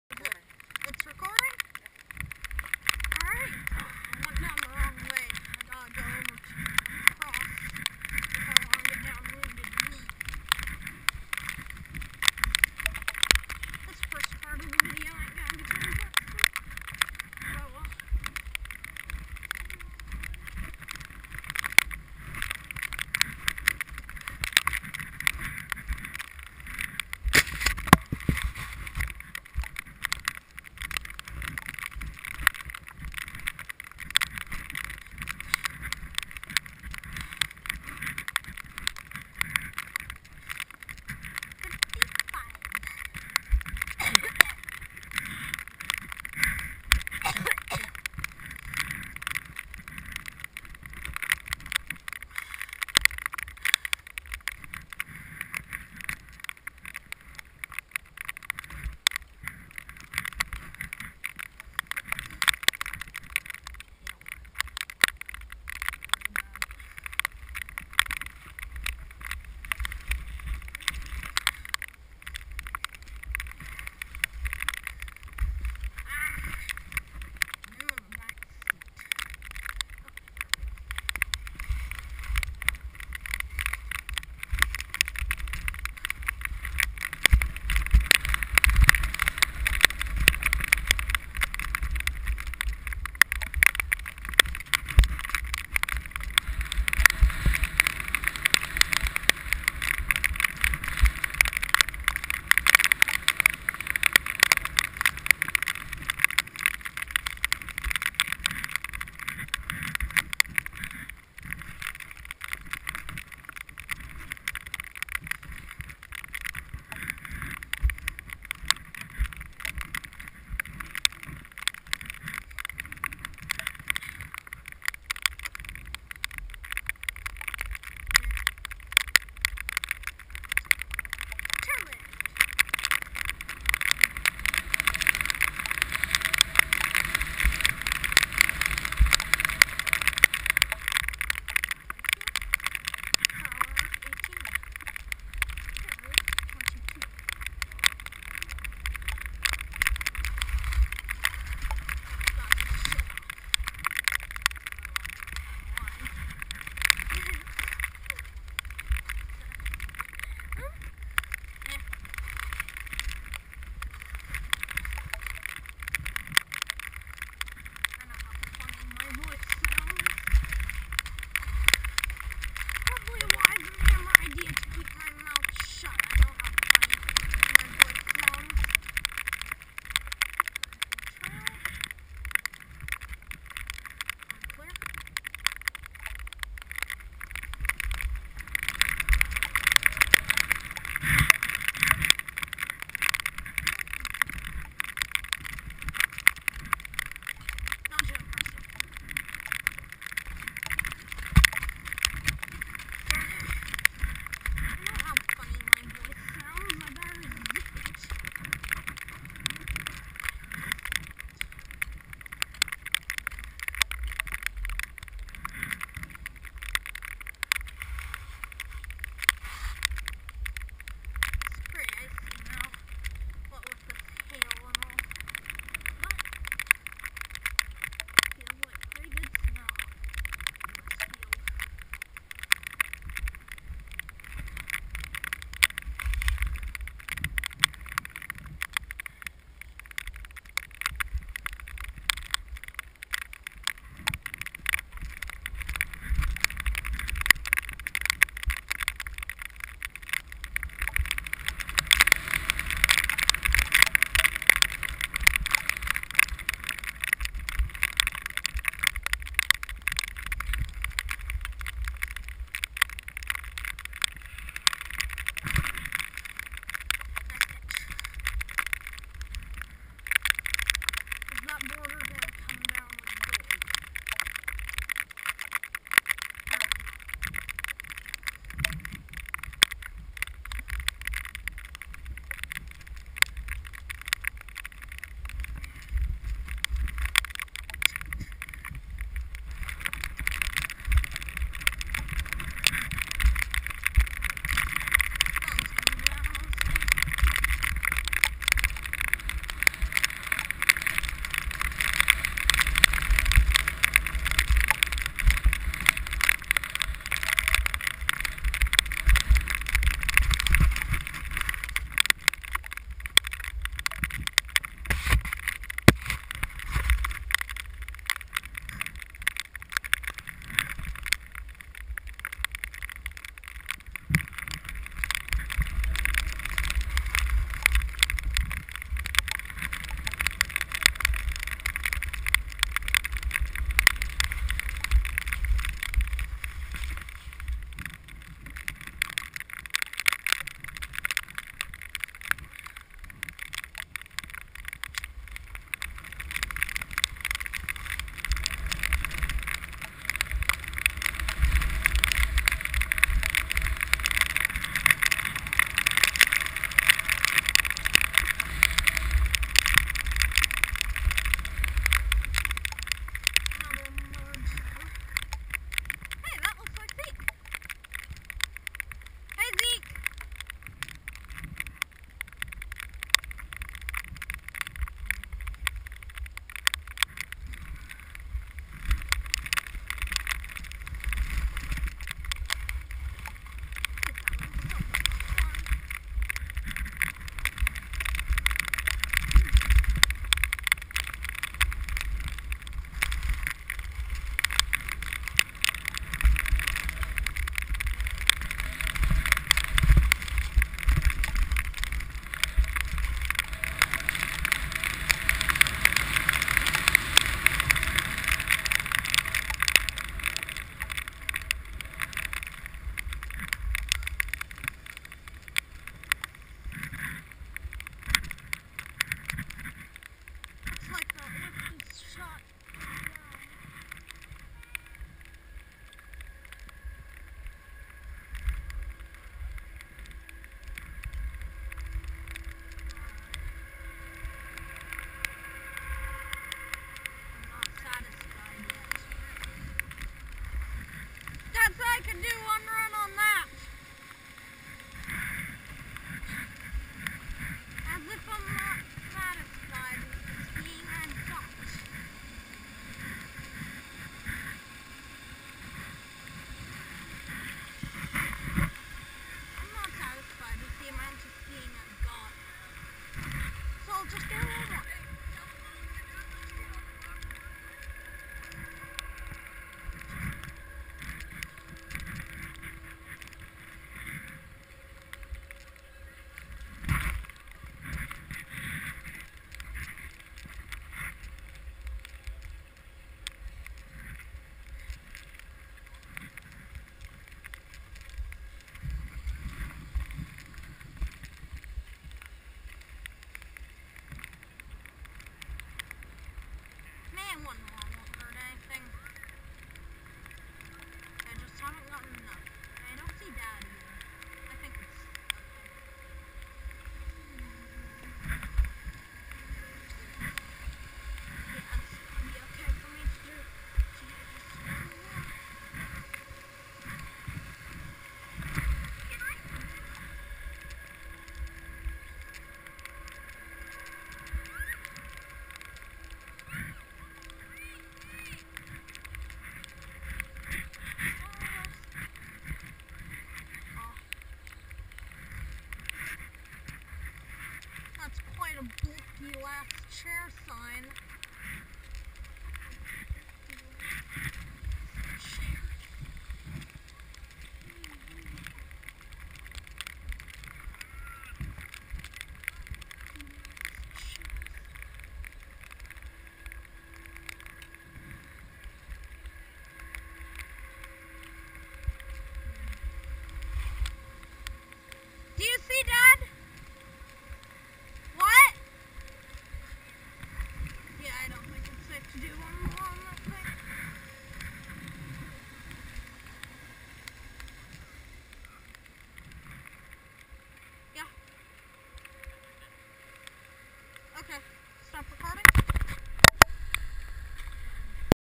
Luke Skiing
Little Luke taking a run with my GoPro